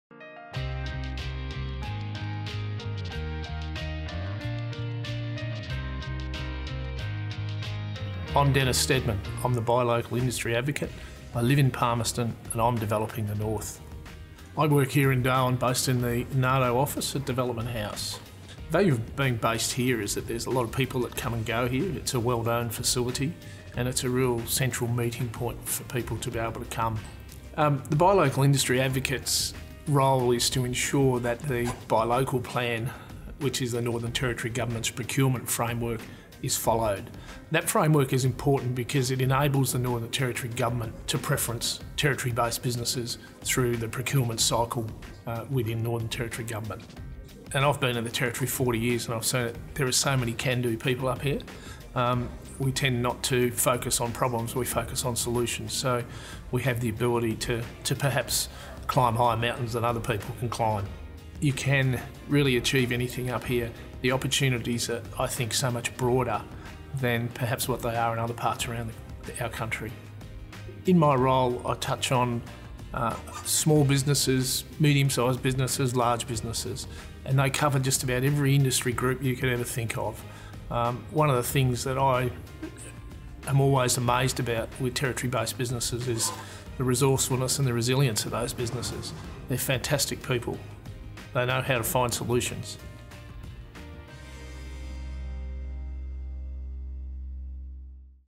NADO Co-located Stakeholder – Meet the Buy Local Industry Advocate
Introducing the Northern Australia Development Office Co-located Stakeholders – Meet Denys Stedman, Buy Local Industry Advocate from Buy Local Industry Advocate Office